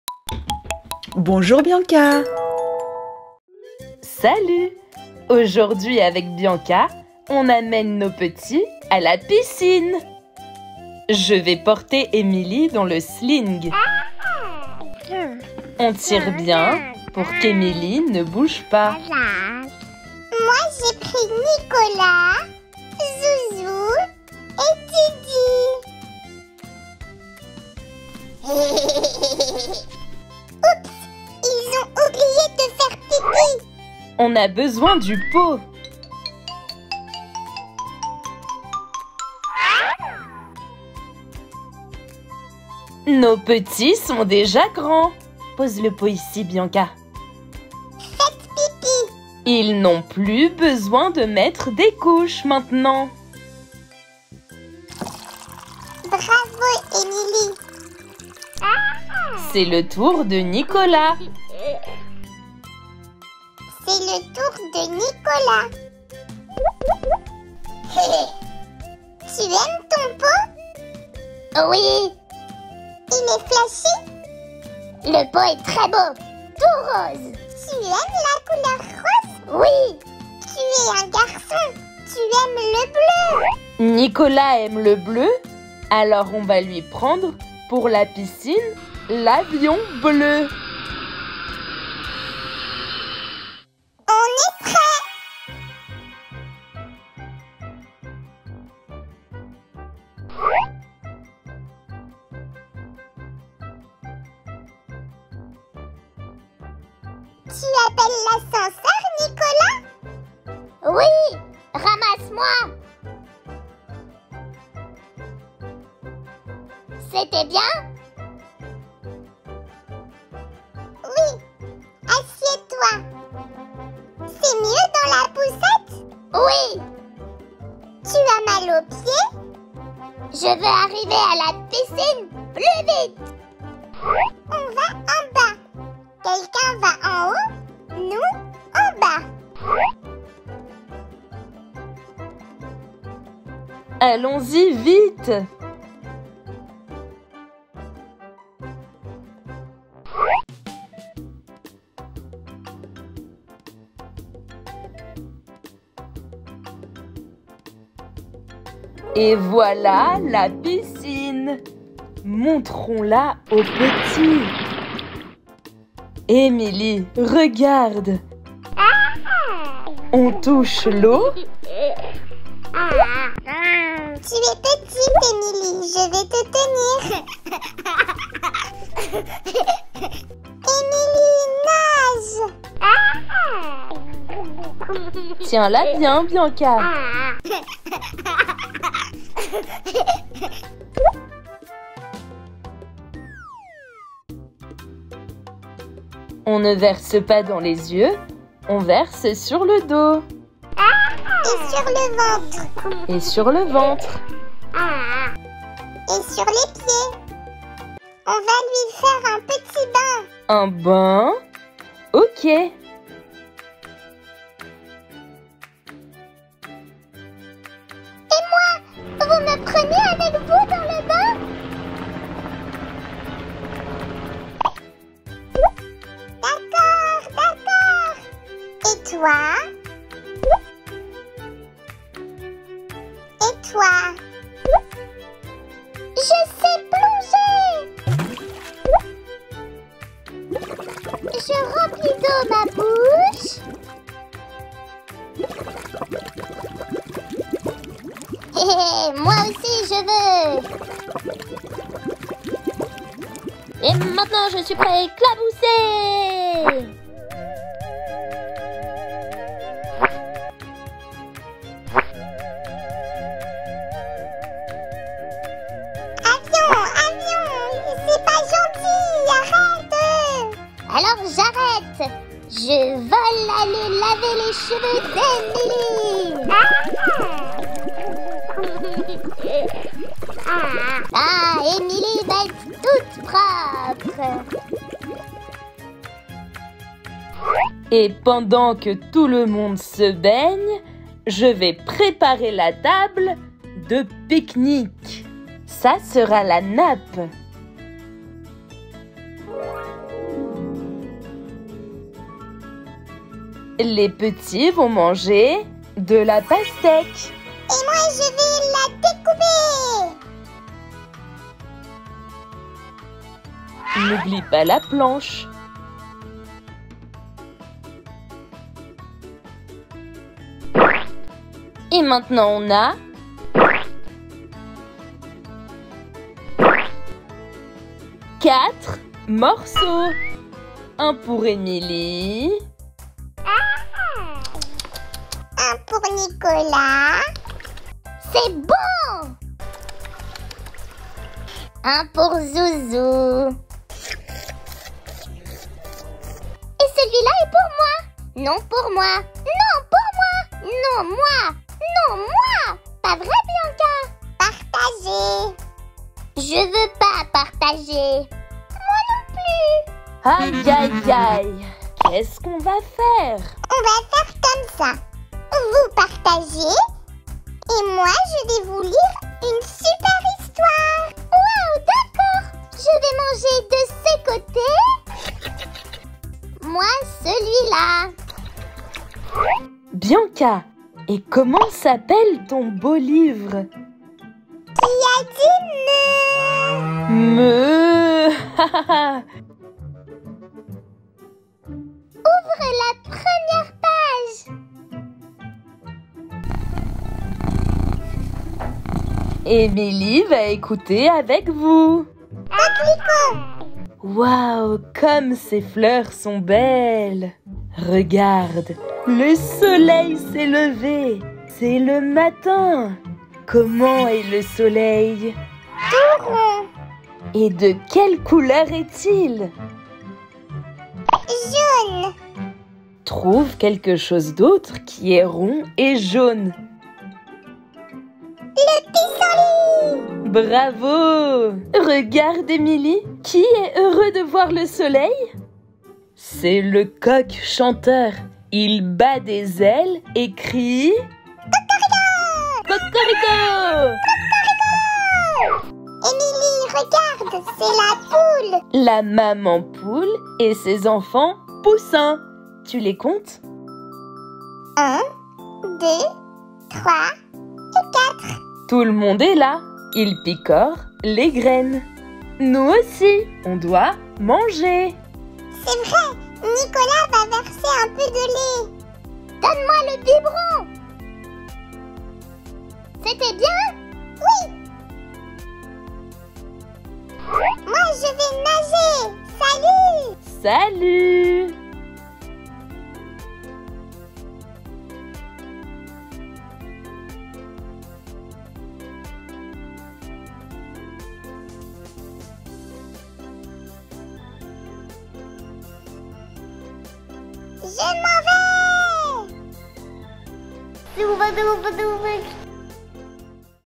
Baby Born et Bianca dans la piscine! Divertissement et éveil pour enfants.
Voici notre nouvelle vidéo de divertissement et d’éveil pour enfants avec Baby Born et Bianca qui nagent et jouent dans la piscine. Bianca et sa maman Maria amènent leurs jouets à la piscine. Maria met Baby Born dans le porte-bébé. Bianca met ses jouets préférés dans la poussette. L’aventure à la piscine commence! La famille va aussi pique-niquer et lire un livre éducatif pour les plus petits.

À bientôt !